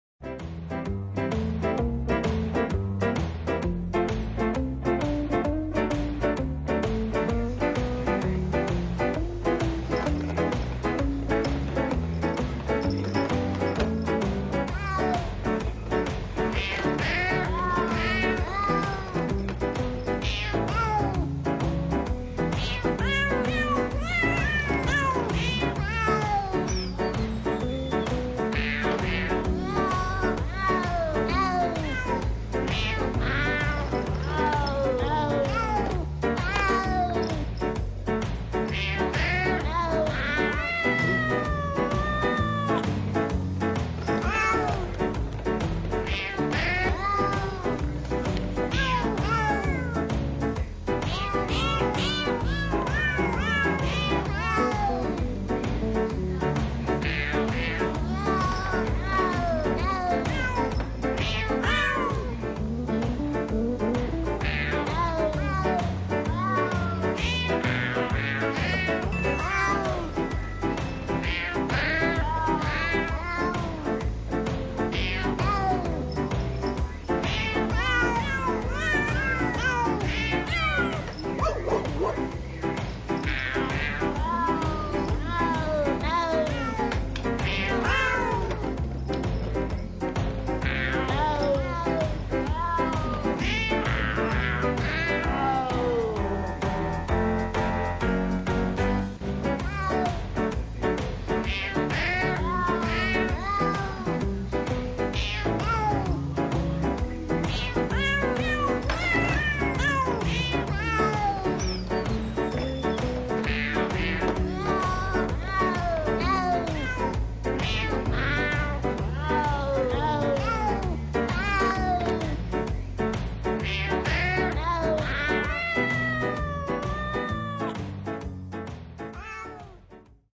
Christmas Toys for Cats
Maggie playing with her new Christmas toy.